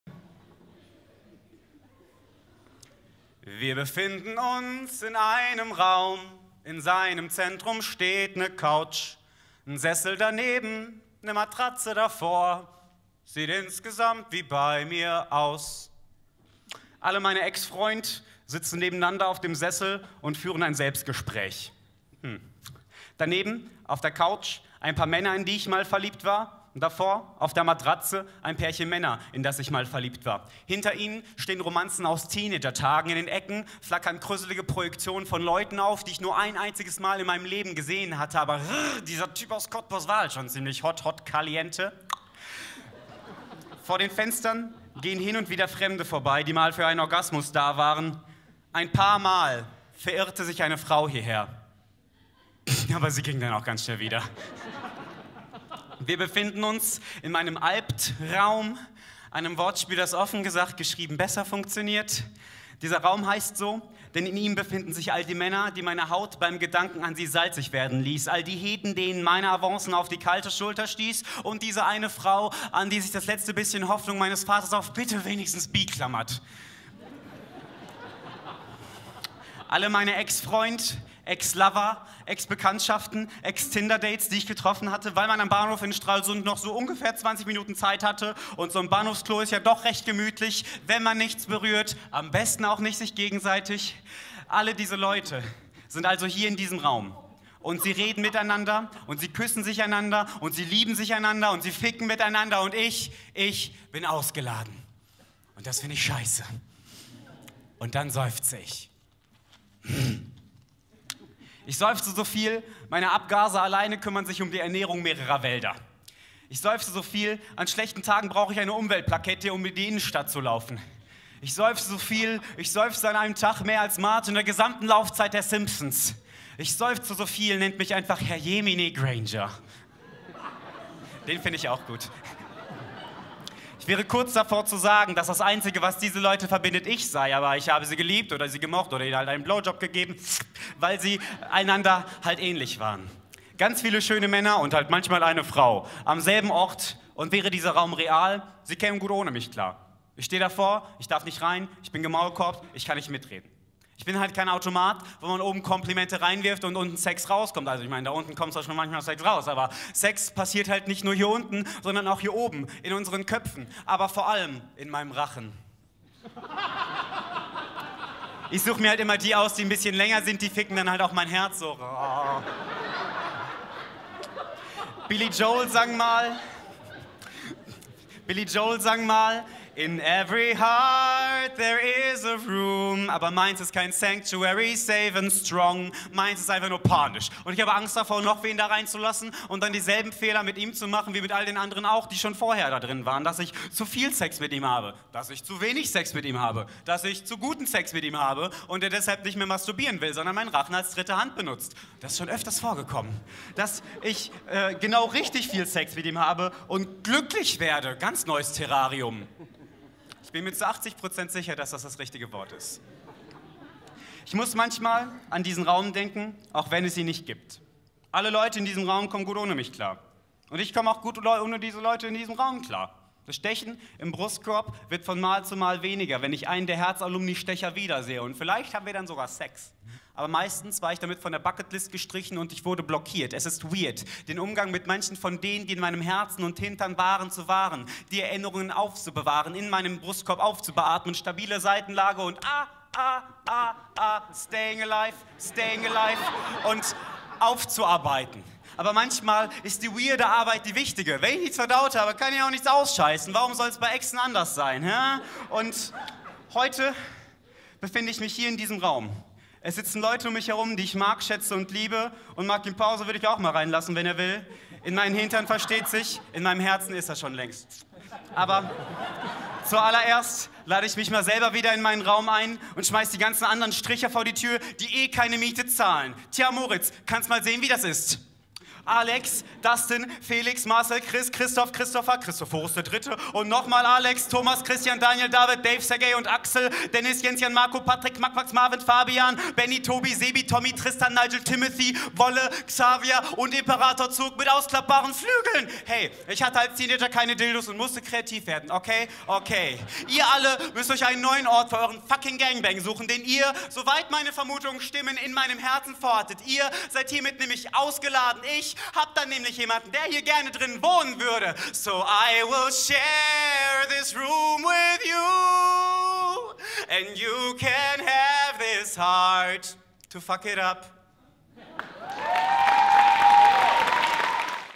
Poesieschlacht | Sven Hensel | And so it goes
Sven Hensel ist Autor, Poetry Slammer und dazu ein gnadenloser Erzähler.

Poesieschlacht, November 2018